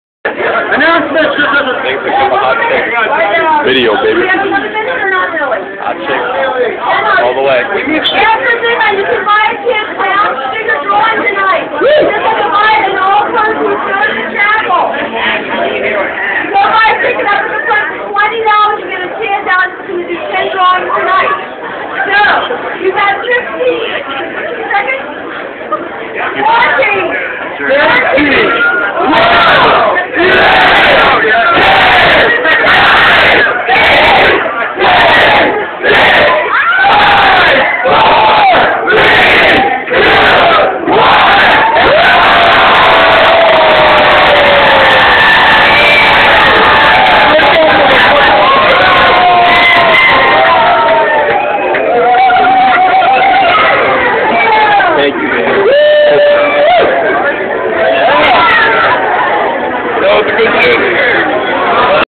Cosm new years 2008
Spent new years at alex greys cosm partying